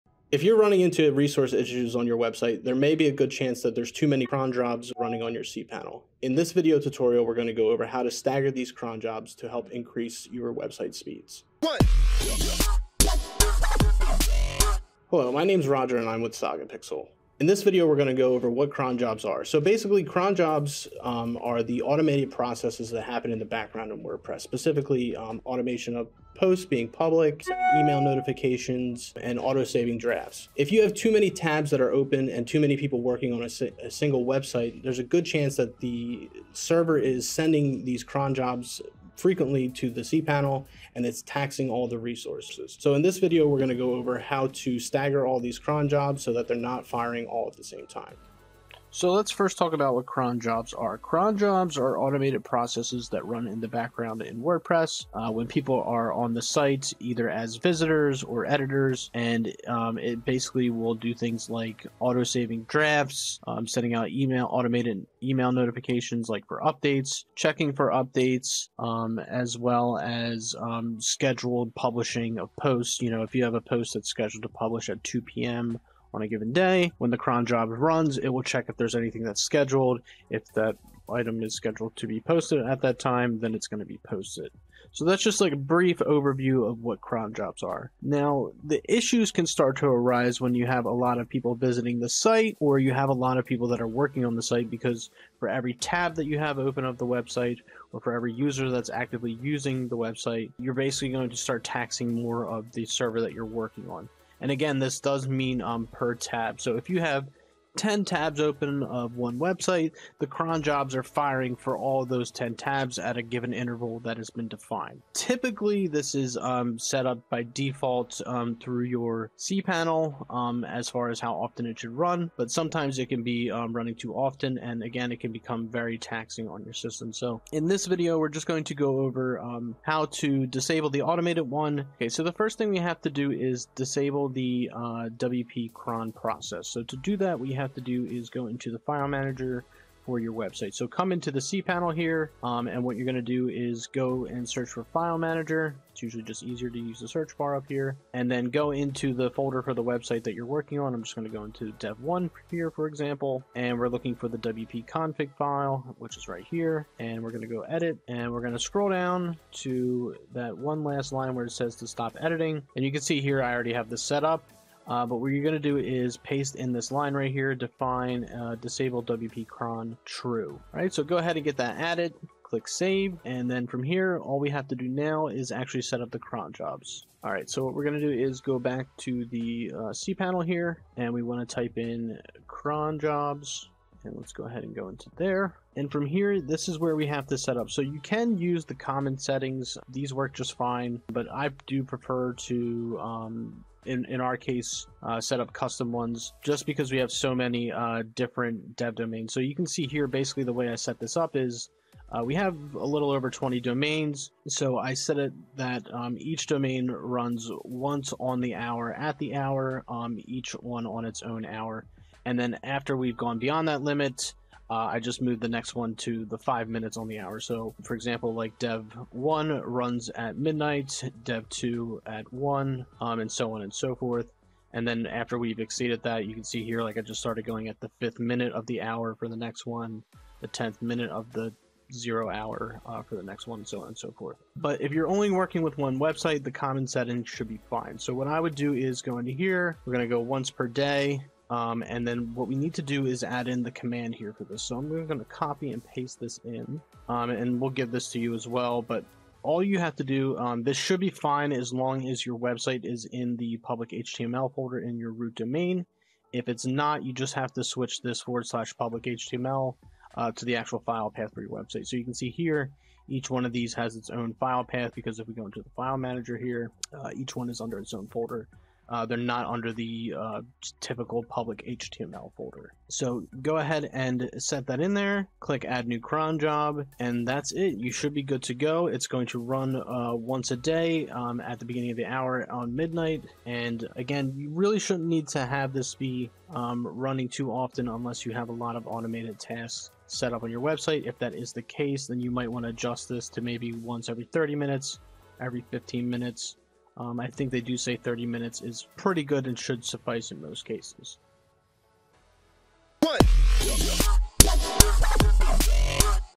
How to Manually Set Cron Jobs Without Plugins
If you have too many cron jobs running on your Cpanel, you may be running into a resource issue across all your websites. In this video, Roger gets into how to stager these corn jobs to help increase your website speed.

Code: cd ${HOME}/public_html; /usr/local/bin/php -q wp-cron.php